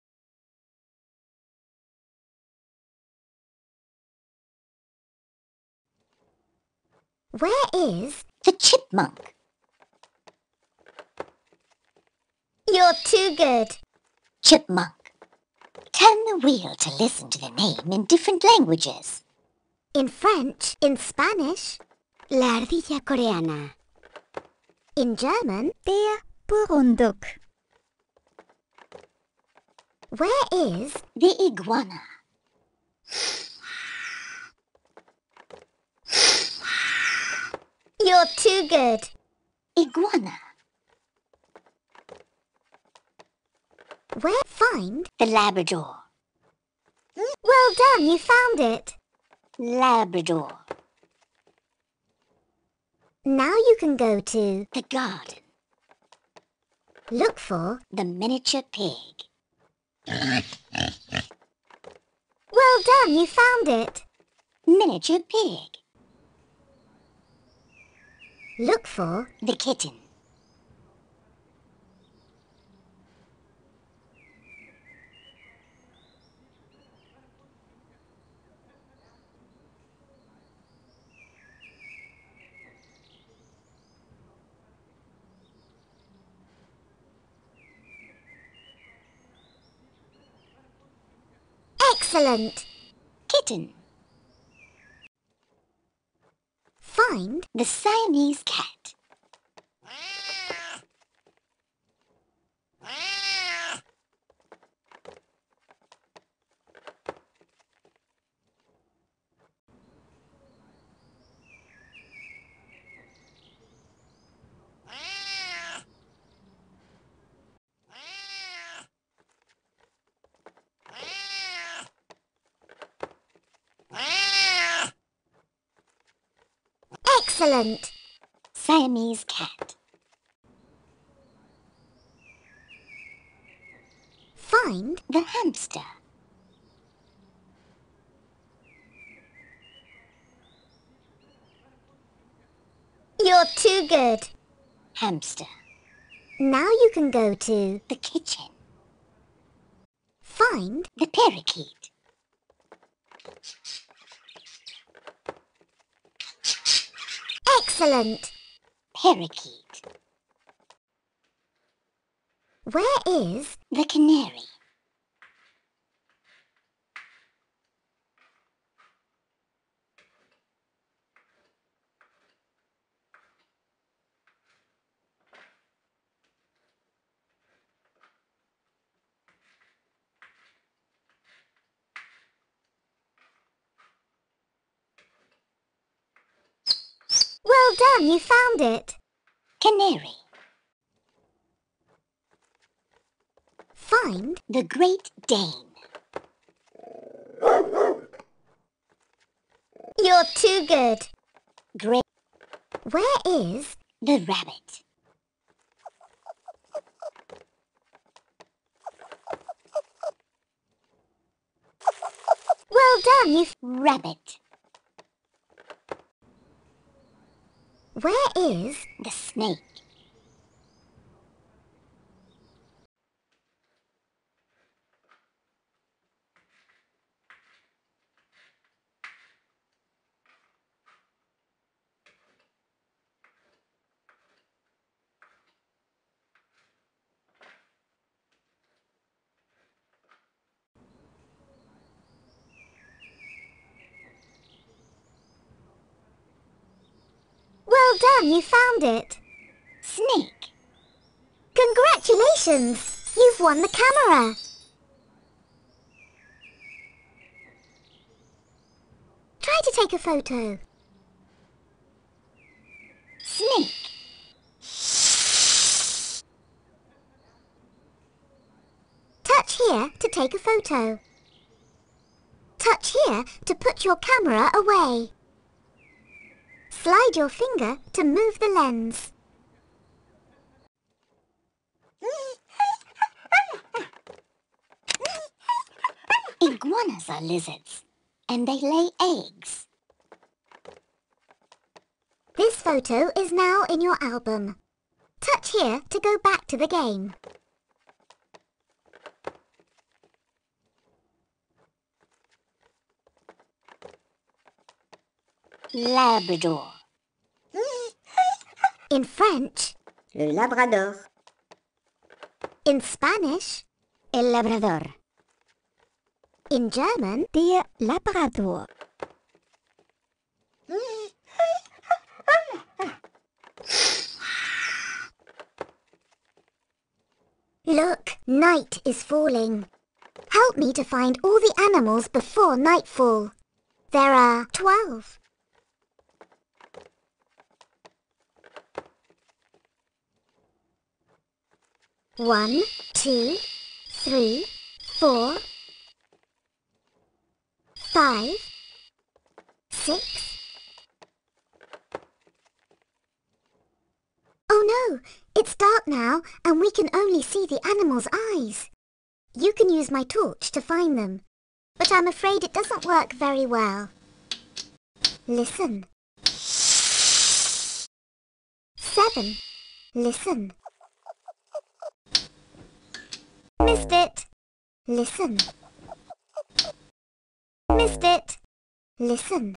Find Them All - Animal Collection | Where is?
This review is of a collection of all things animals - from pets to worldwide creatures. Each app is loaded with animal info for kids 2-8 years old.